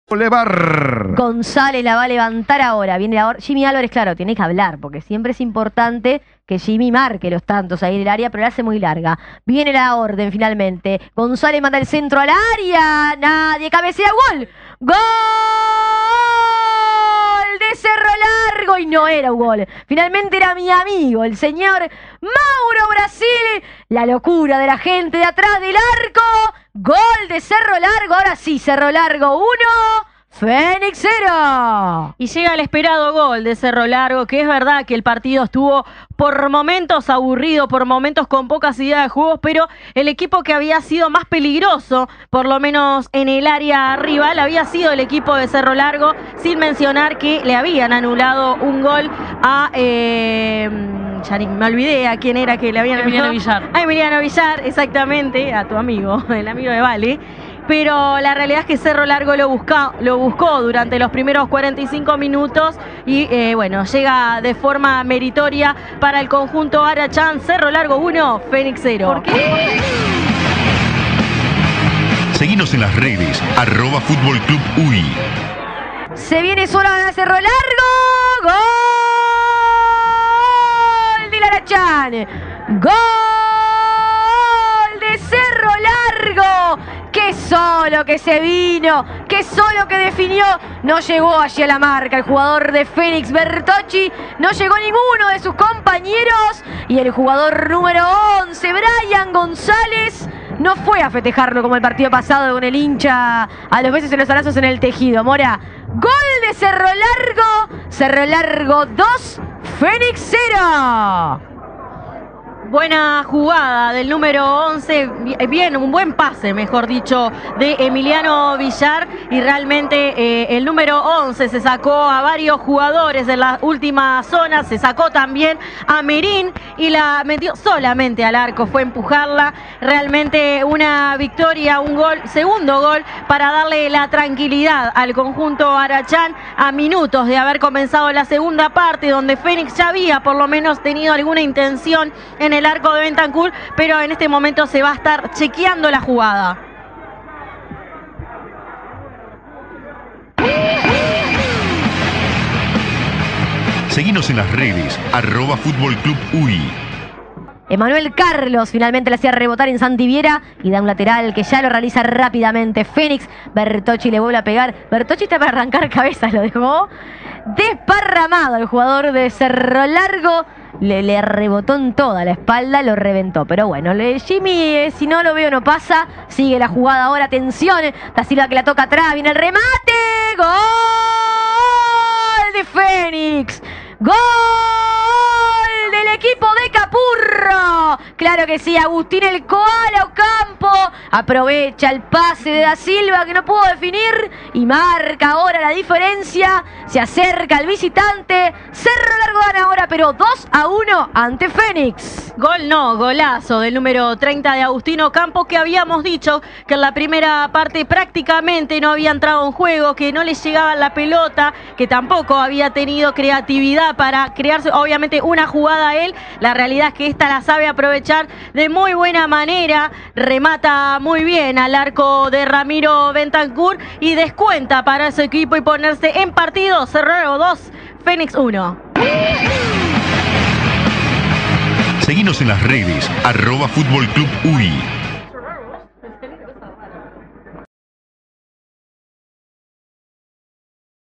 Cerro Largo FC 2 Fénix 1 Intermedio 2023 (por Fiorella Pose)
Los goles del partido, por Fiorella Pose y Sofía Mora para Fútbol Club por CX 40 Radio Fénix 1330 AM.